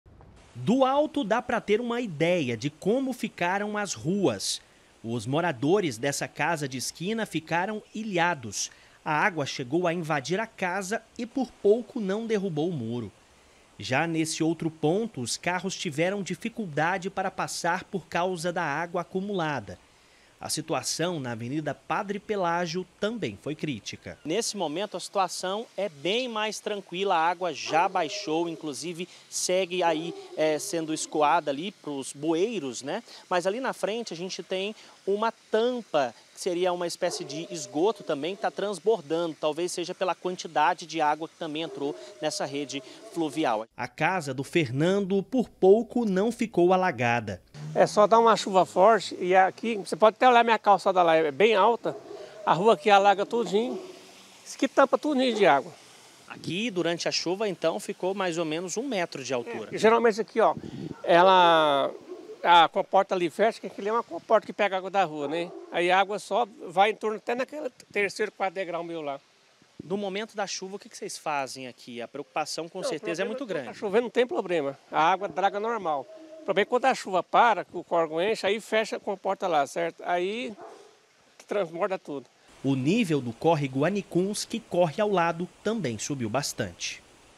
RUAS ALAGADAS E PREJUÍZOS COM CHUVA EM GOIÂNIA
A CHUVA QUE ATINGIU GOIÂNIA FEZ CÓRREGOS TRANSBORDAREM, DEIXOU RUAS ALAGADAS E CAUSOU PREJUÍZOS. OS ALAGAMENTOS AFETARAM DIVERSAS REGIÕES, COMO A AVENIDA PADRE PELÁGIO E O SETOR SÃO JOSÉ.